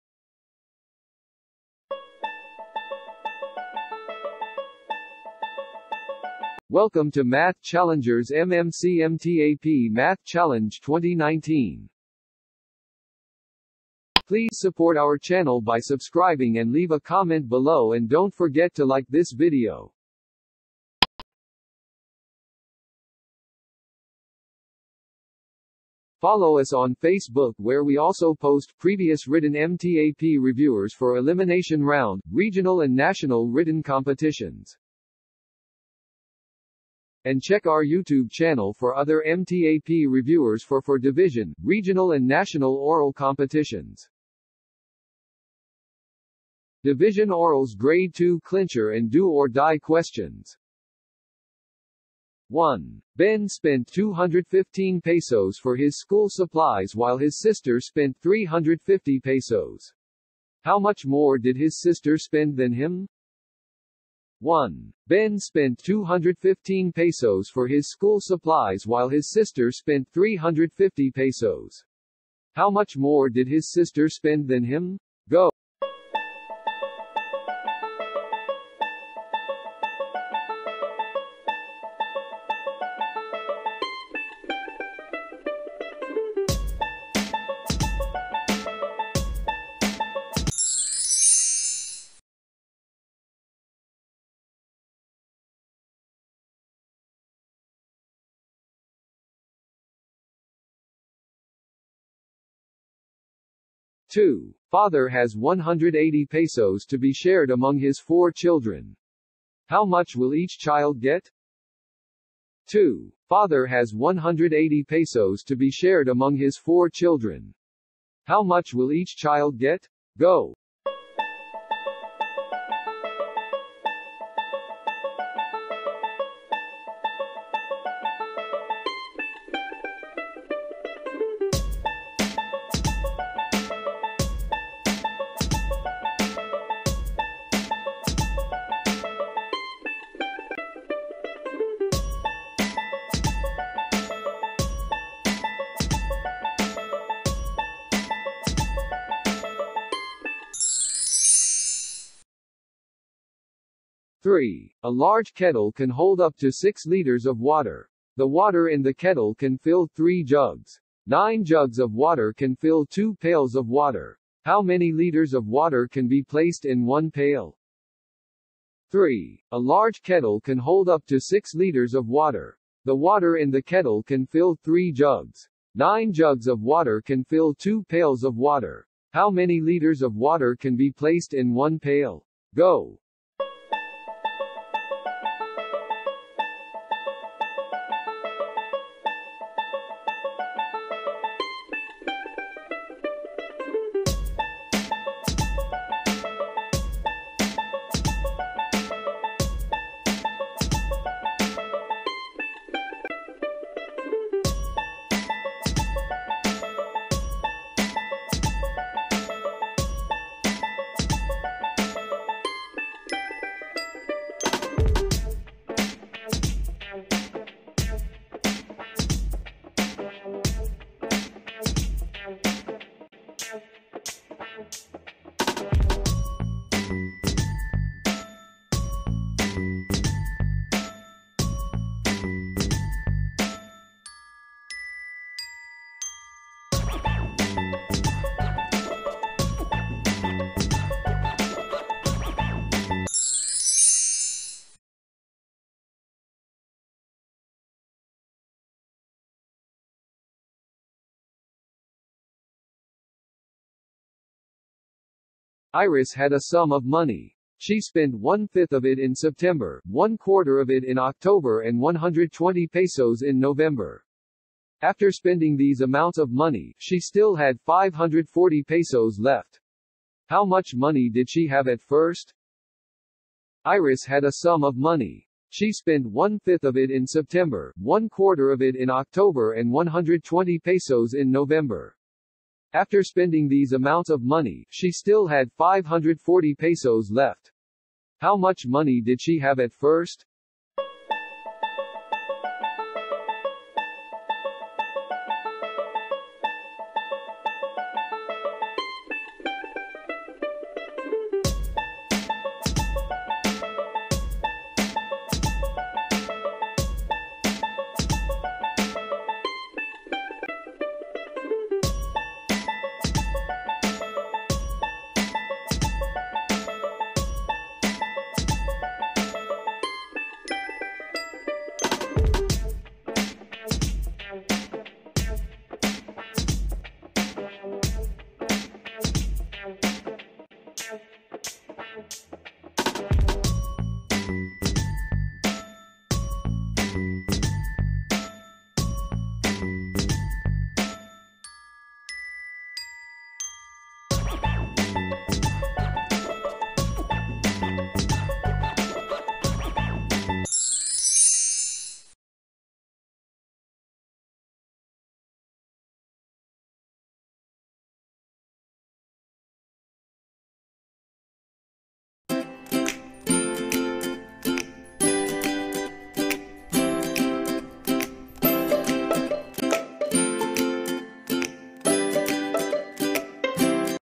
Grade 2 - Clincher and do or die Round 2019 MTAP-MATH CHALLENGE Division Orals
MMC MTAP-MATH CHALLENGE 2019 Division Orals- Grade  2